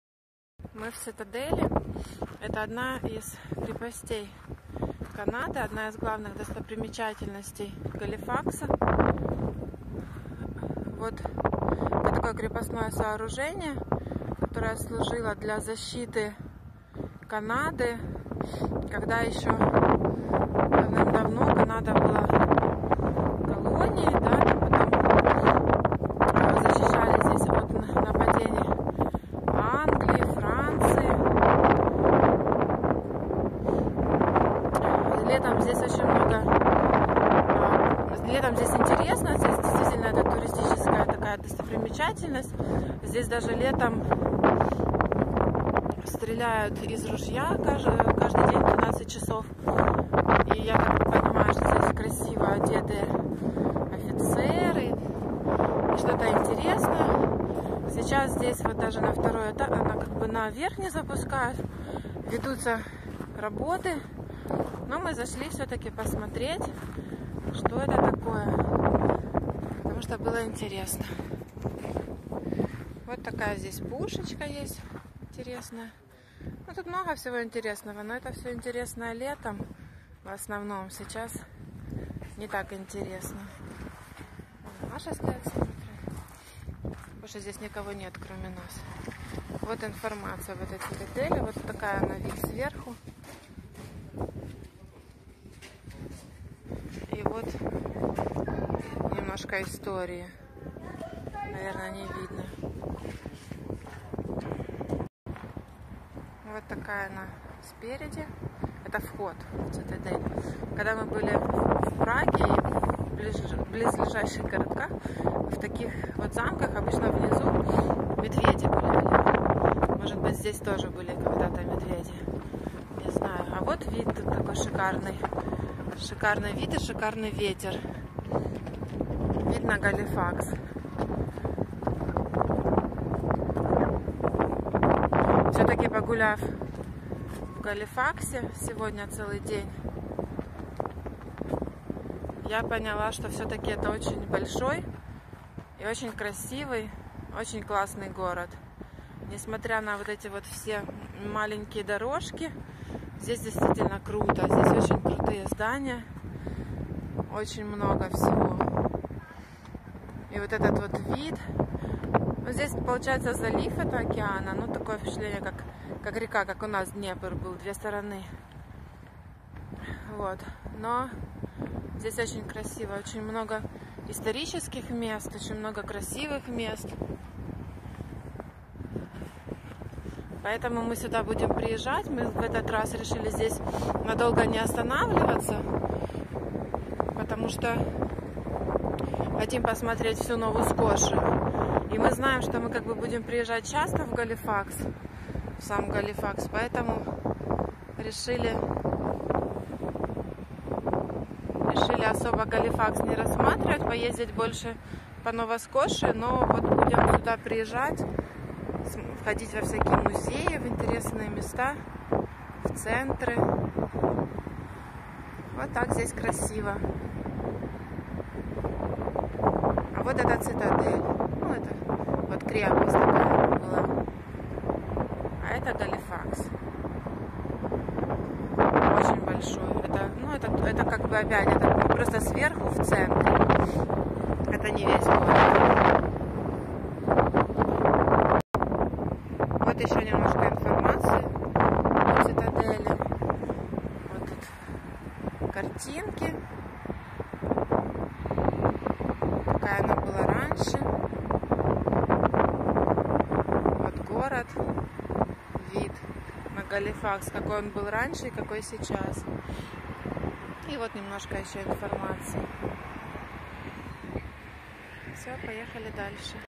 Историческая крепость в Галифаксе. Город с холма.
Одна из достопримечатеьностей не только Галифакса, но и Канады в целом. Эту устрашающего вида крепость в форме звезды строили с 1828 по 1856 год для защиты подступов к городу как со стороны суши, так и с моря. Это четвертая цитадель, которую построили здесь, и одна из самых часто посещаемых достопримечательностей Канады. Крепость охраняет стража в шотландских юбках, в полдень здесь производят торжественные залпы из пушки, а также в музее проходят интересные выставки, например, демонстрация шоу «Волны истории». Это все летом, а сейчас - просто старинное здание